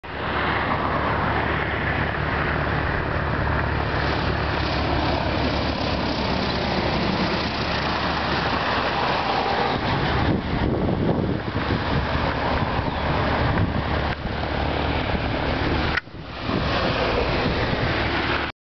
Cessna 172 landing at SCTB (Aeródromo Tobalaba / Tobalaba Airfield, Santiago)
Not sure about the model. Please correct me if necessary.